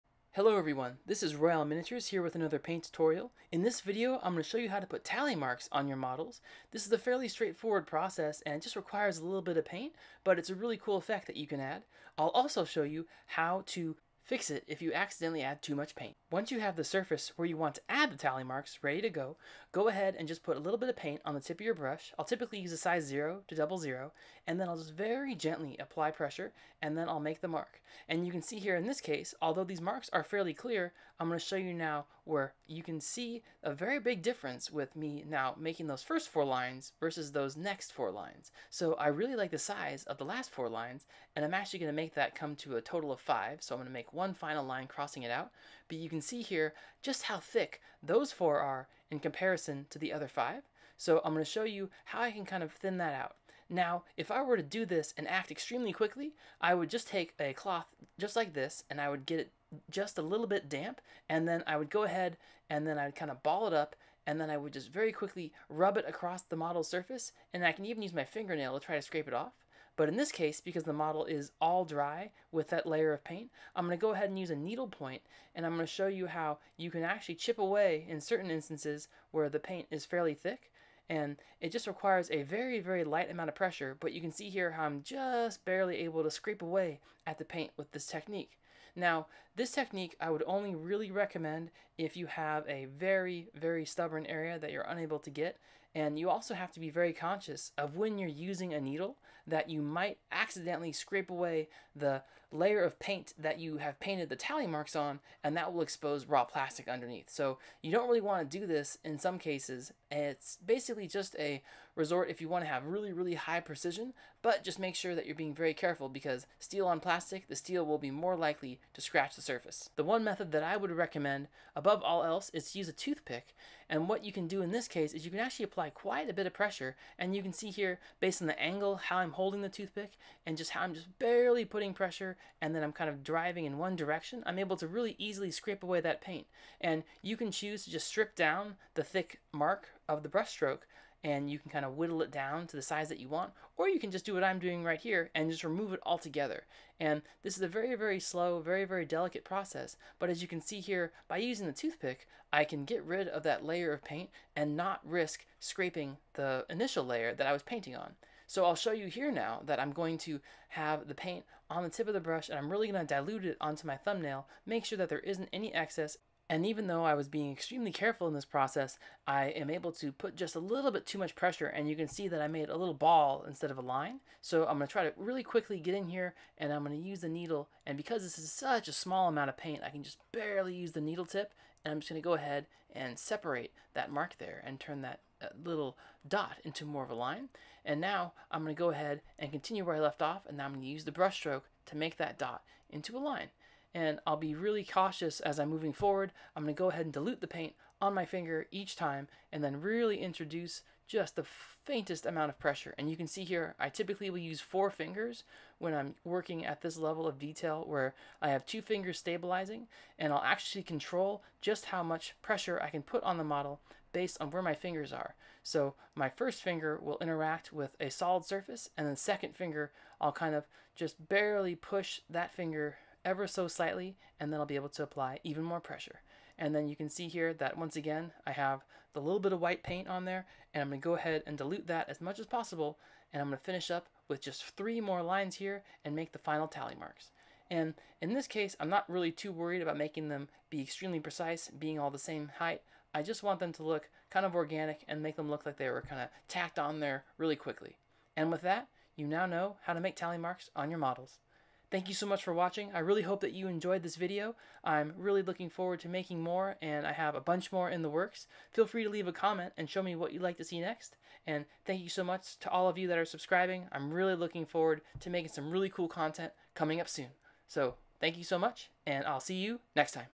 Painting Tally Marks
Want to add tally marks to your models?  See how to paint these small details and ways you can fix mistakes if you accidentally add too much paint!

Hello Everyone!

This tutorial continues from my Painting Custom Sword Decals video.  If you want to see how the model got to this stage, watch the link below on YouTube.

- Royale Miniatures